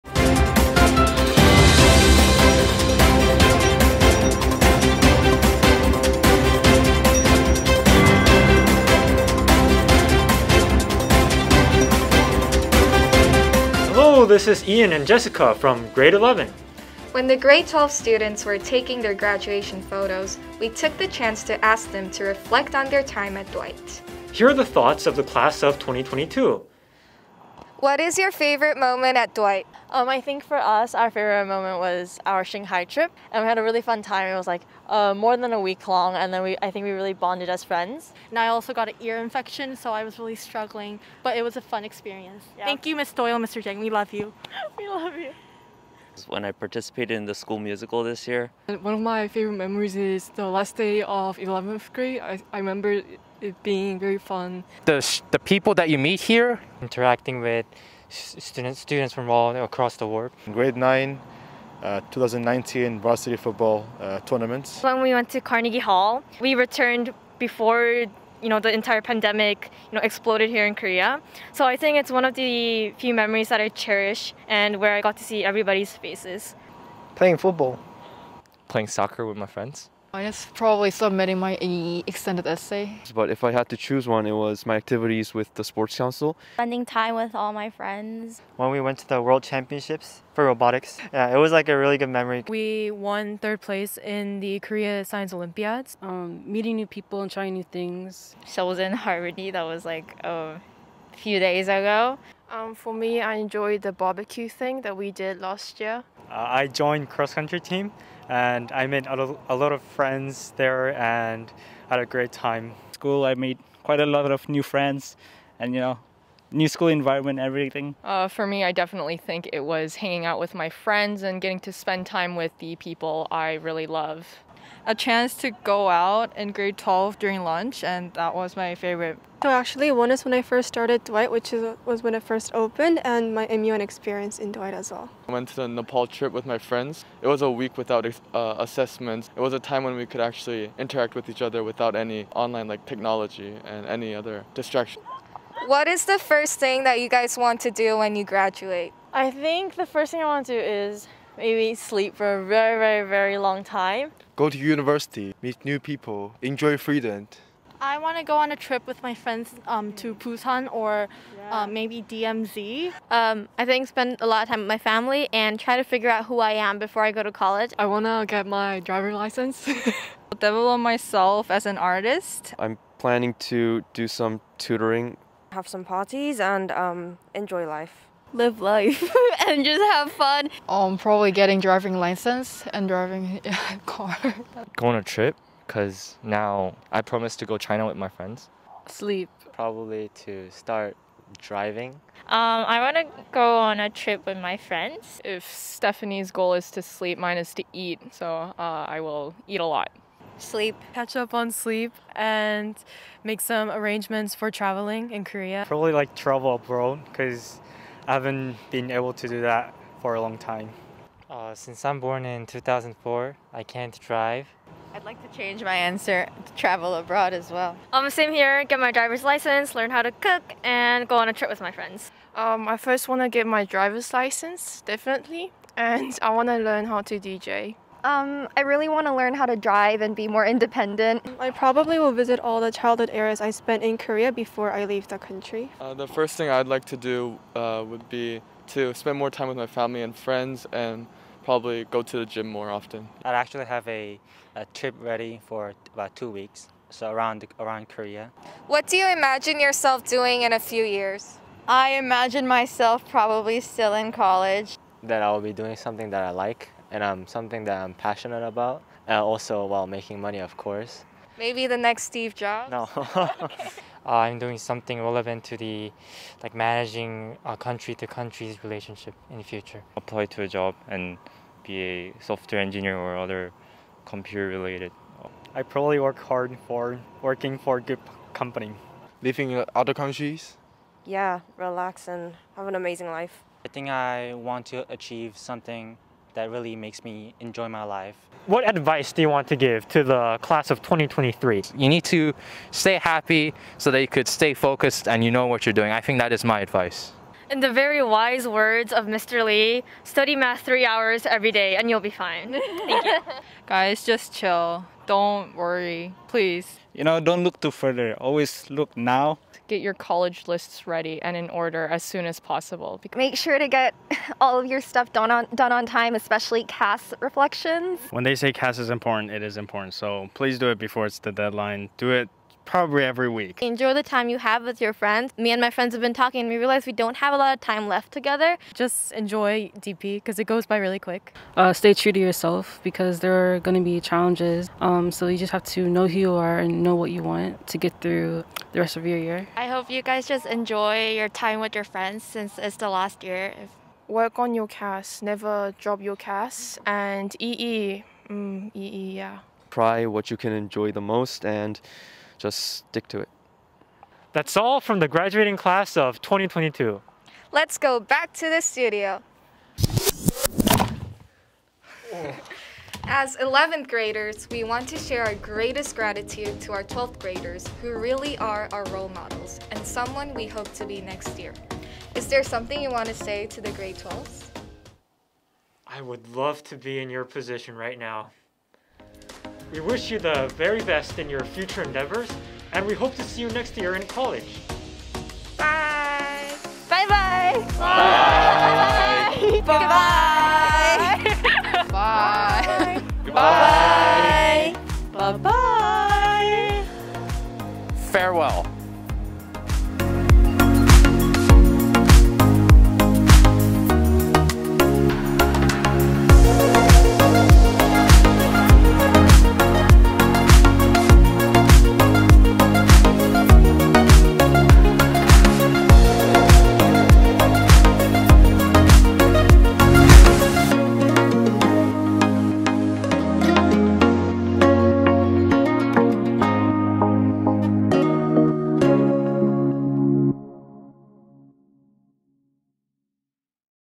DSS News Special: Class of 2022
Hear the final thoughts of the Class of 2022, as they speak with DSS News presenters Jessica and Ian from Grade 11.